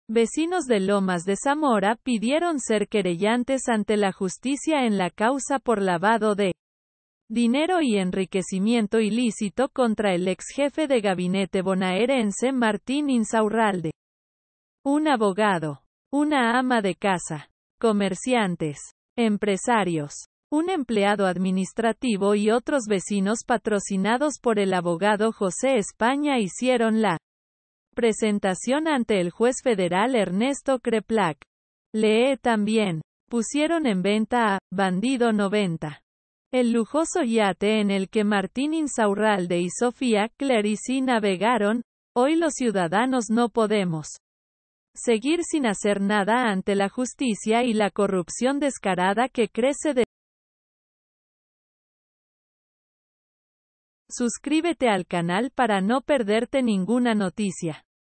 Un grupo de vecinos Lomas de Zamora pidieron ser querellantes en la causa contra Insaurralde
Vecinos de Lomas de Zamora pidieron ser querellantes ante la Justicia en la causa por lavado de dinero y enriquecimiento ilícito contra el ex jefe de Gabinete bonaerense Martín Insaurralde.
Un abogado, una ama de casa, comerciantes, empresarios, un empleado administrativo y otros vecinos patrocinados por el abogado José España hicieron la presentación ante el juez federal Ernesto Kreplak.
Leé también: Pusieron en venta a “Bandido 90″, el lujoso yate en el que Martín Insaurralde y Sofía Clérici navegaron“Hoy los ciudadanos no podemos seguir sin hacer nada ante la Justicia y la corrupción descarada que crece desesperadamente en nuestro país”, advirtieron en el escrito presentado.
“Estos querellantes pretenden actuar como observadores del proceso lo que constituye a aumentar la transparencia de la investigación, máxime teniendo en cuenta el descreimiento de la Justicia”, argumentó en su escrito el abogado.
El tramo desde Buenos Aires hasta el aeropuerto de La Romana no baja de los U$S60 mil.